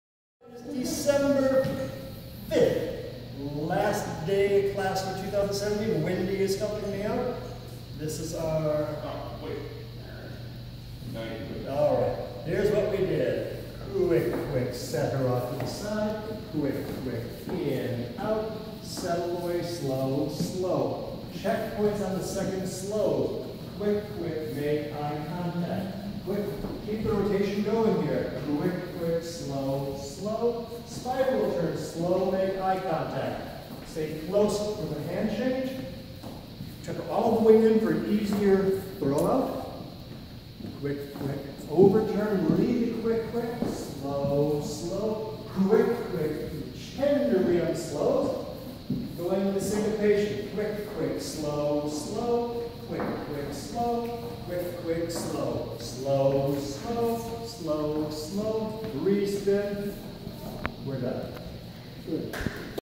countrydance5678int
Dec 5, 2017 730 class. Finished pattern with synchopations.